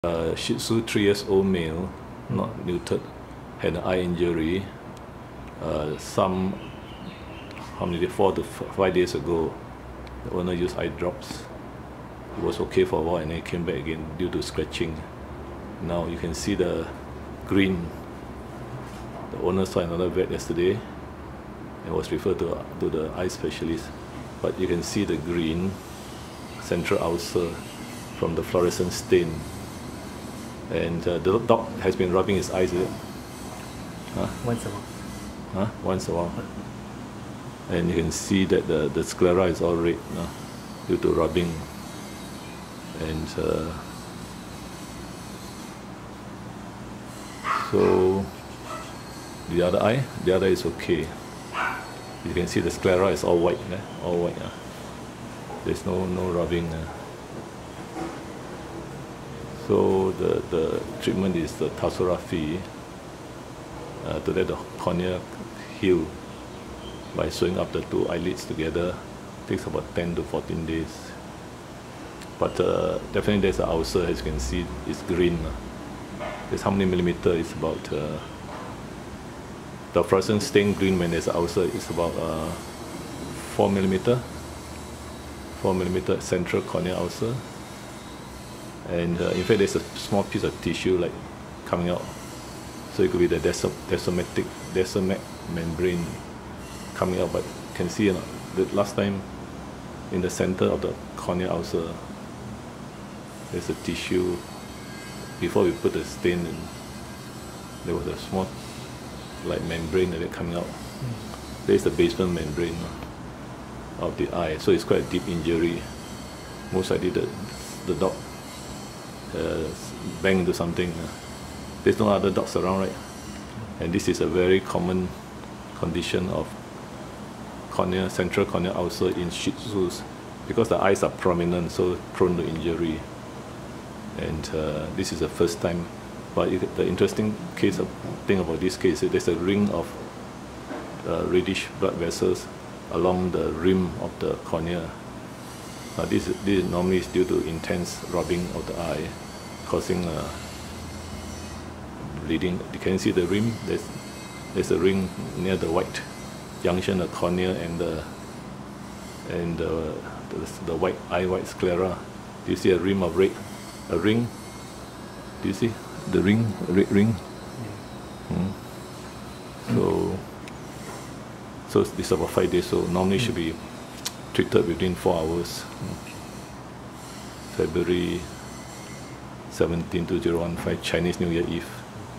A Shih Tzu has a painful eye ulcer - descemetocoele
Feb 17, 2015. Four days ago, this 3-year-old Shih Tzu had a painful left eye. A vet referred the owner to one of two vet specialising in eye problems. She prescribed rimadyl tablets for 4 days. She recorded that the eye was negative to fluorescein stain test.

But the vet specialist would only give an appointment some 7 days later. The other specialist also had a long waiting list. He consulted me.

This dog's eye  left eye is not so painful on Feb 17, 2015 when I saw him. There is a positive fluorescein stain test. A descemetocoele can be seen.

Corneal eye injuries are emergencies and require prompt vet treatment within 4 hours of injury.

Tarsorrhaphy is a treatment. The dog also had a red ring of inflammation around the rim of corneal-scleral junction inside the eye.

Feb 17, 2015 is the eve of the eve of Chinese New Year but many corneal ulcerations cannot wait for several days for treatment. In this case, the dog must have been rubbing his left eye vigorously, causing a descemetocoele as the first vet's finding was "no fluorescein" stain, meaning no corneal ulceration.